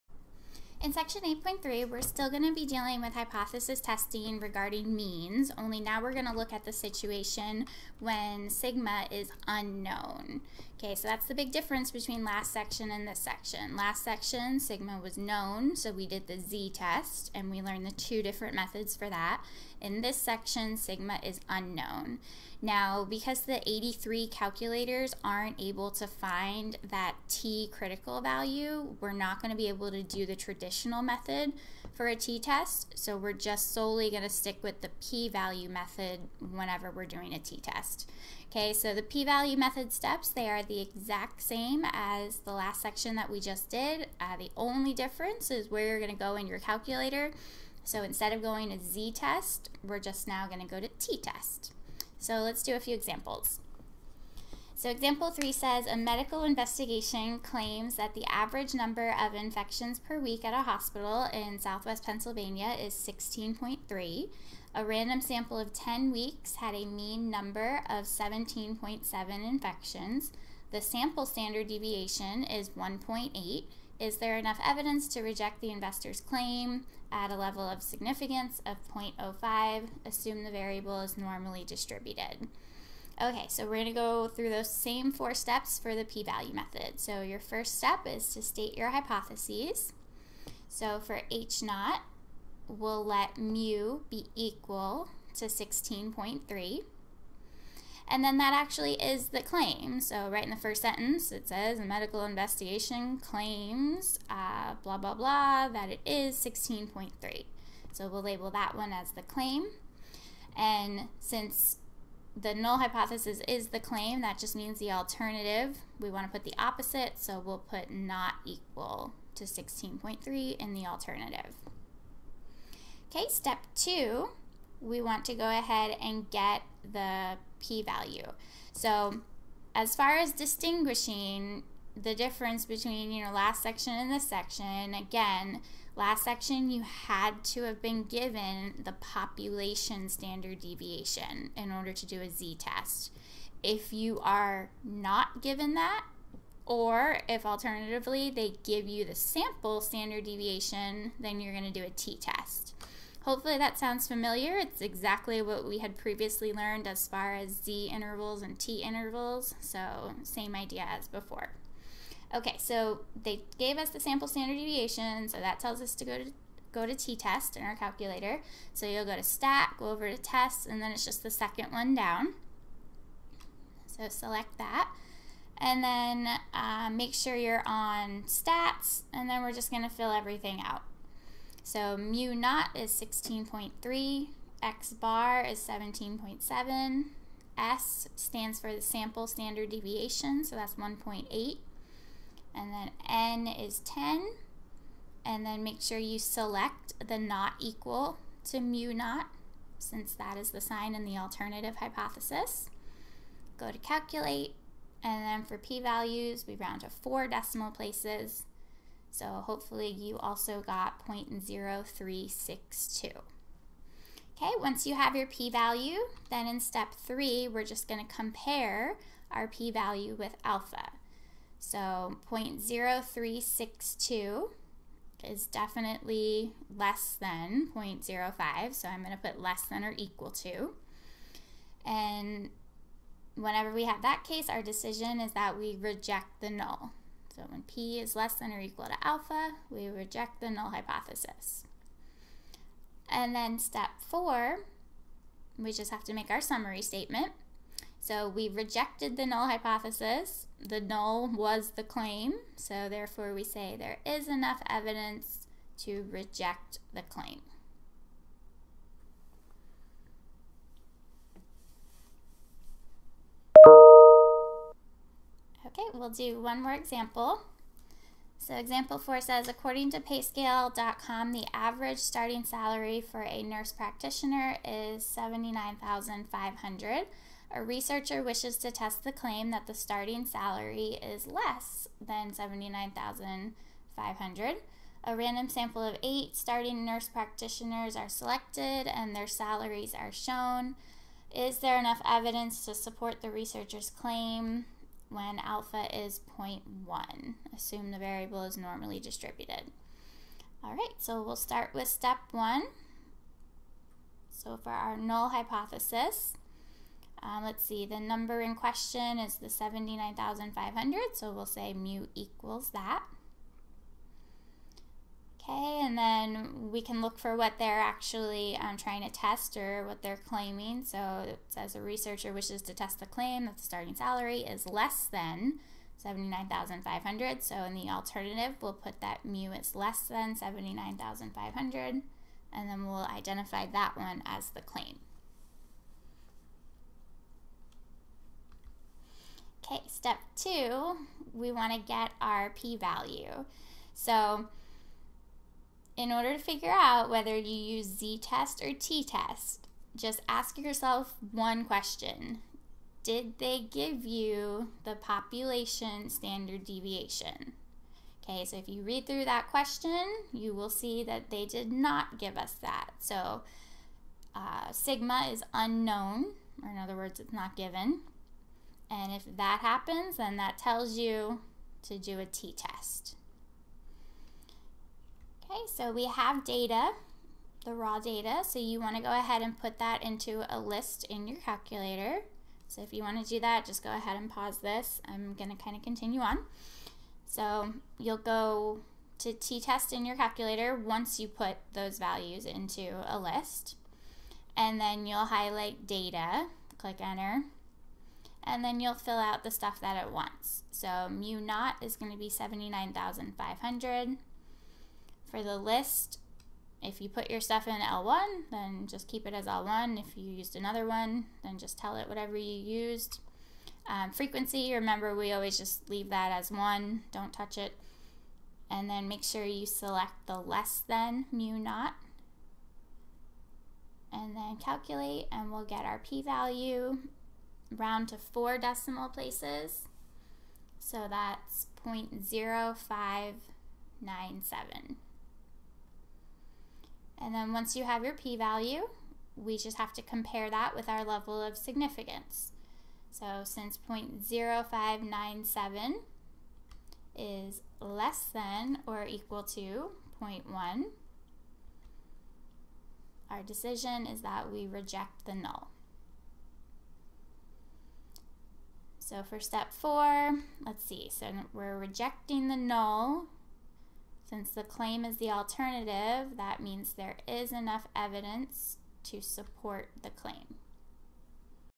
Statistical Methods (STA 2023) - Section 8.3 - The P-value Method for a t-test
In this video, Karen explains how to do The P-value Method for a t-test when hypothesis testing about a mean.

Key Points:

1. Use a t-test when the population standard deviation is unknown.

2. We will only do the p-value method when it comes to t-tests. (Compared with z-tests, where we learned two methods and will use them both.)